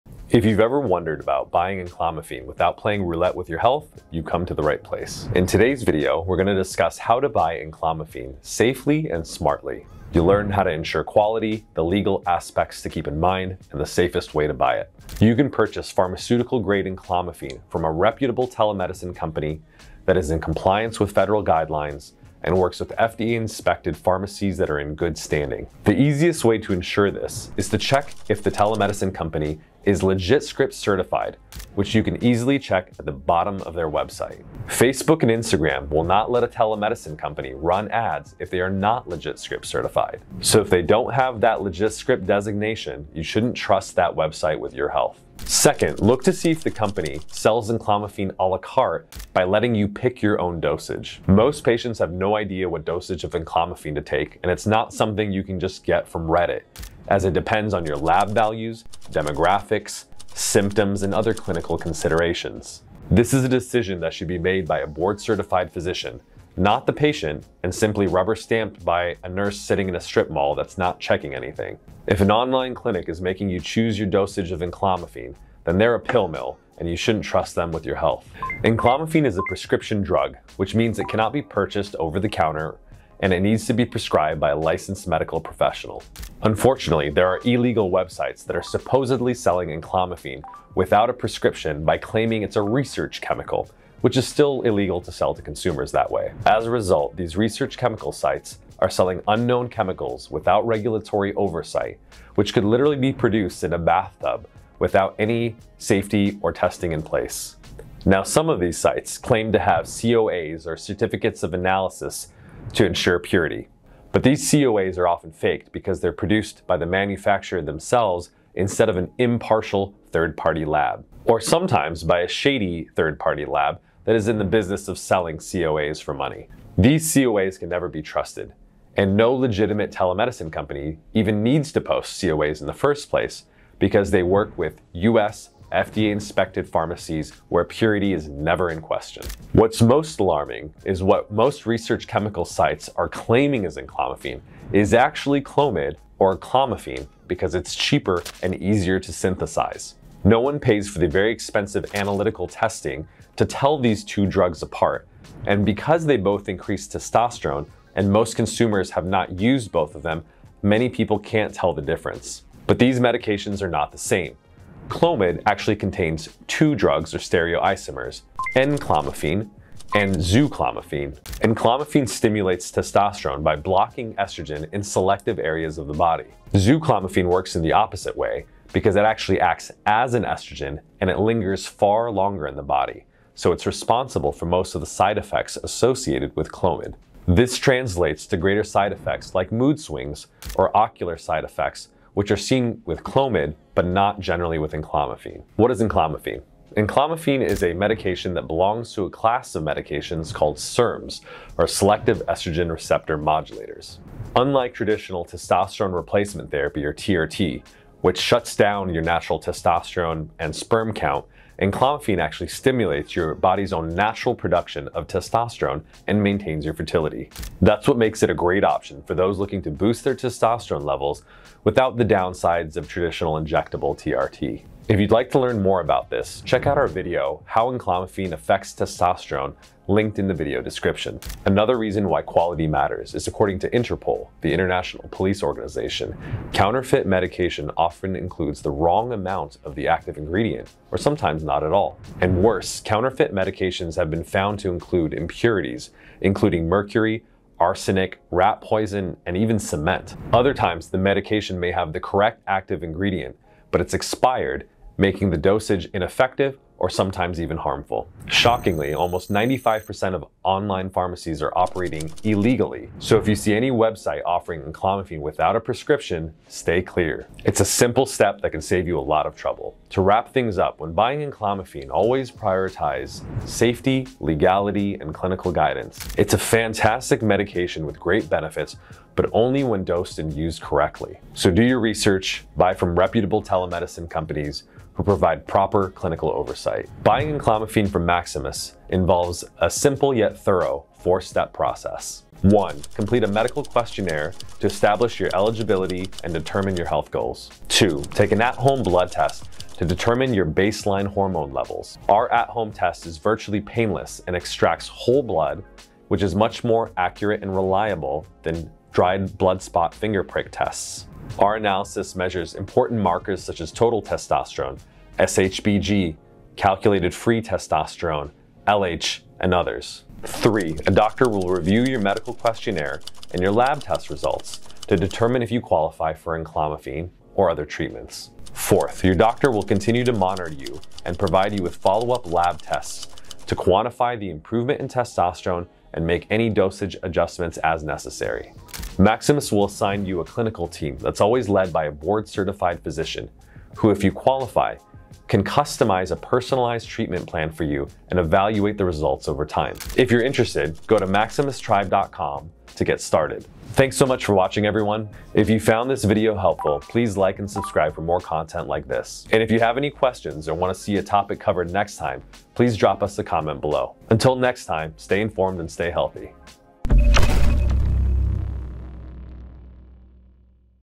How To Buy Enclomiphene Online: Safety, Avoiding Fake Medication, Dosing & More.
In today's video Dr. Cam breaks down how to buy enclomiphene online safely and legally. In the world of hormone optimization, enclomiphene has quickly risen to popularity; proving itself to be clinically significant way to increase testosterone levels in men.

Video timestamps:
0:00 - 0:27 - Introduction
0:28 - 0:44 - Check For LegitScript Certification
0:45 - 1:17 - Check to see how enclomiphene is dosed.
1:18 - 1:47 - Avoiding Fake Enclomiphene
1:48 - 3:14 - Fake Certificates Of Analysis & Enclomiphene
3:15 - 3:51 - What is Enclomiphene?
3:52 - 4:29 - Why quality matters
4:30 -  4:50 - Recap
4:51 - 5:49 - Buying Enclomiphene With Maximus
5:50 - 6:10 - Subscribe for more content!